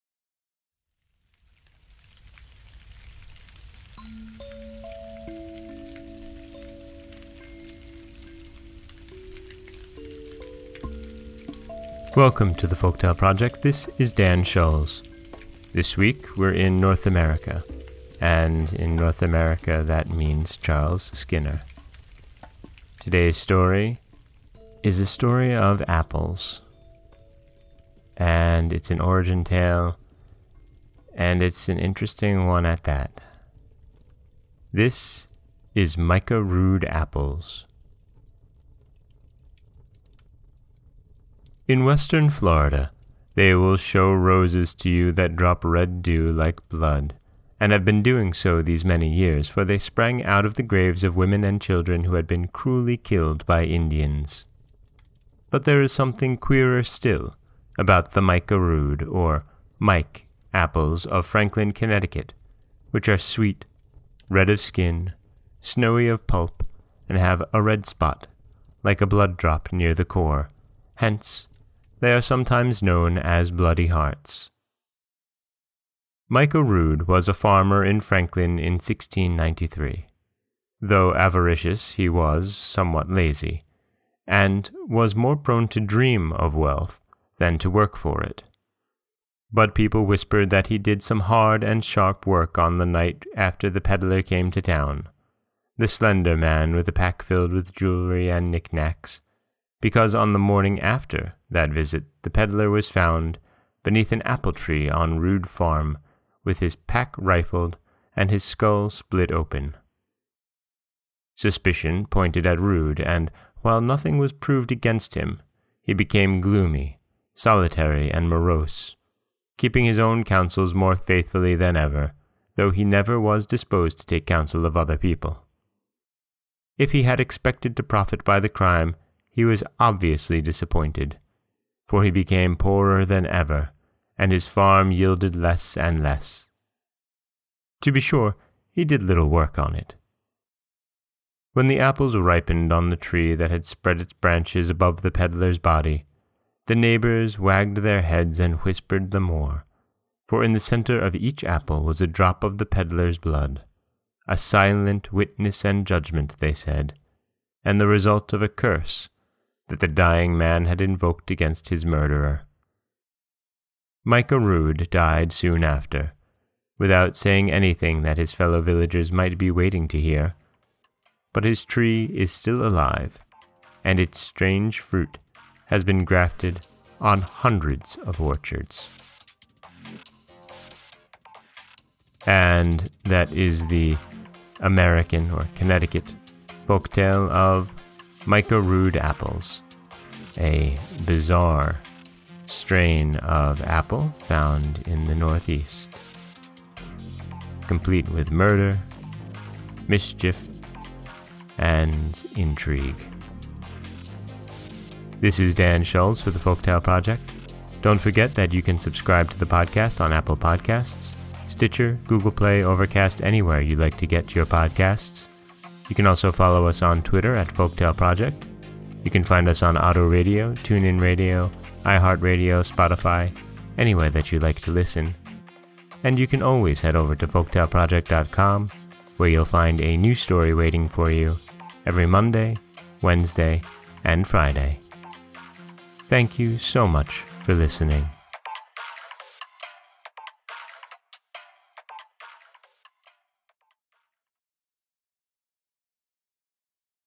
Micah Rood Apples
In Western Florida they will show roses to you that drop red dew, like blood, and have been doing so these many years, for they sprang out of the graves of women and children who had been cruelly killed by Indians. But there is something queerer still about the Micah Rood—or “Mike”—apples of Franklin, Connecticut, which are sweet, red of skin, snowy of pulp, and have a red spot, like a blood-drop, near the core; hence they are sometimes known as bloody-hearts.&nbsp;